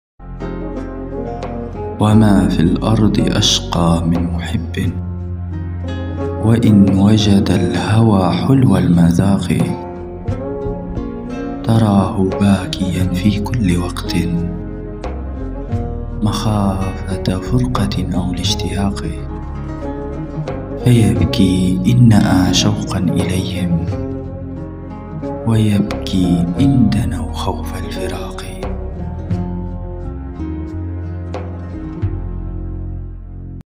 وما في الارض أشقى من محبٍ // إبن دريد الازدي قصيدة فصحى بصوت الوسمي عبدالله
وَما في الأَرضِ أَشقى مِن مُحِبٍّ

وَإِن وَجَدَ الهَوى حُلوَ المَذاقِ

تَراهُ باكِياً في كُلِّ وَقتٍ

مَخافَةَ فُرقَةٍ أَو لِاِشتِياقِ

فَيَبكي إِن نَأى شَوقاً إِلَيهِم

وَيَبكي إِن دَنَوا خَوفَ الفِراقِ